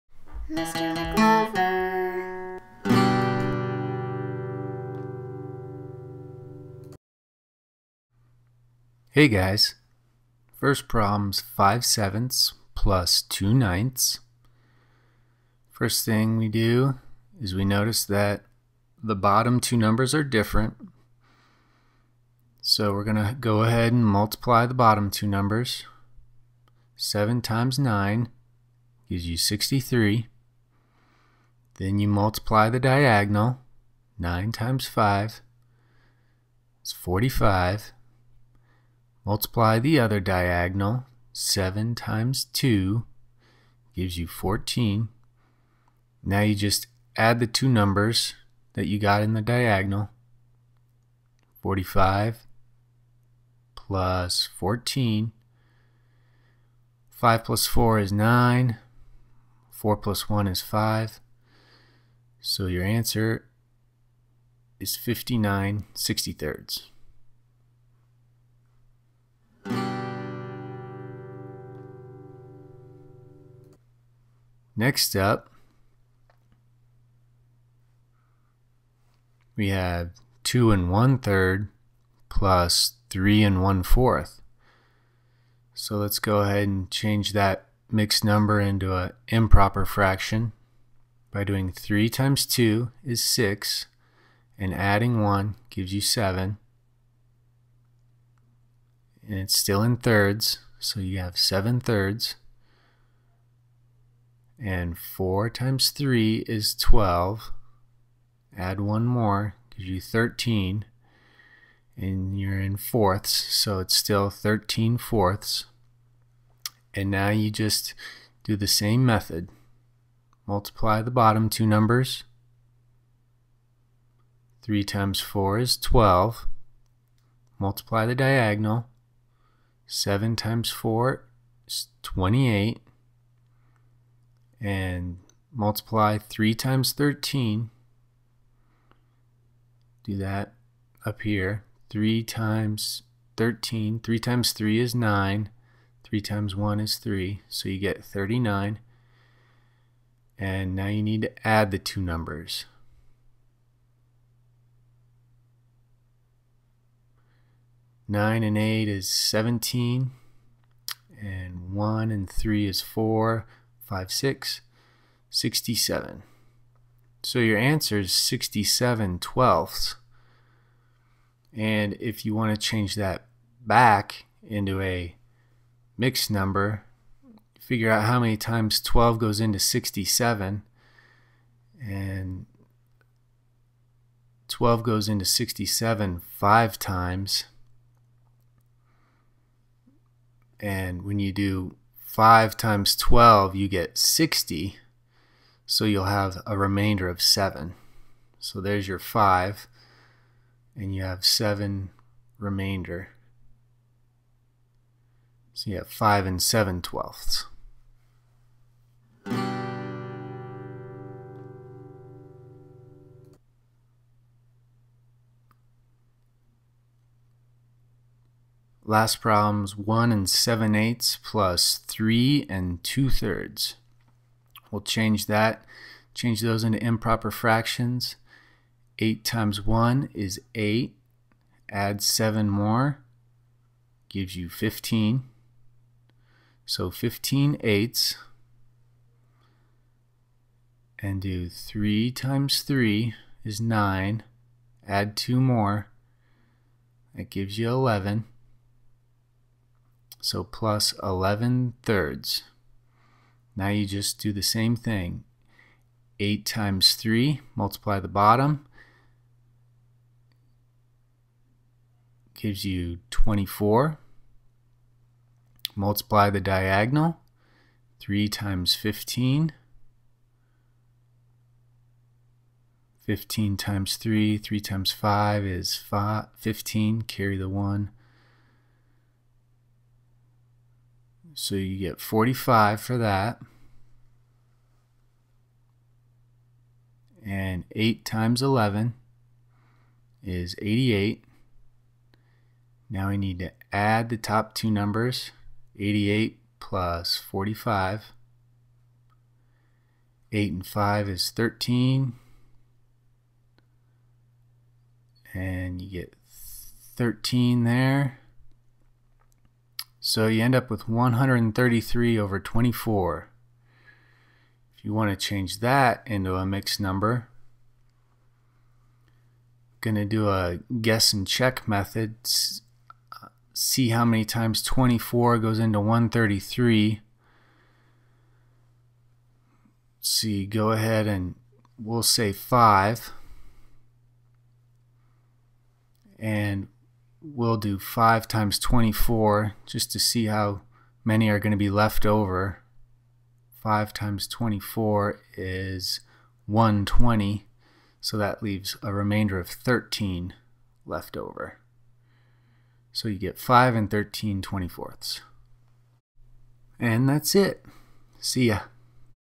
Add Fractions (Mixed Numbers) With Unlike Denominators Part 2
Fractions, mixed numbers, addition!

This method is for students that want to solve the problem the same way every time. This method will produce the correct answer every time, but the student may have to simplify the fraction once finished. Nonetheless, at least the student can produce the correct answer every time!

Note: Any mixed numbers have to be changed to improper fractions first.
1) Multiply the denominators (bottom numbers). This is the denominator in your answer.
2) Multiply the diagonals. Place the answers above the fractions (see video).
3) Add the numbers you placed above the fractions (this is the numerator of your answer [top number]).

Another Note: You can use this method for subtraction of fractions. To do that, subtract the numbers in step 3 instead of adding them.

Mr McGlover makes math and reading resources for children and teachers.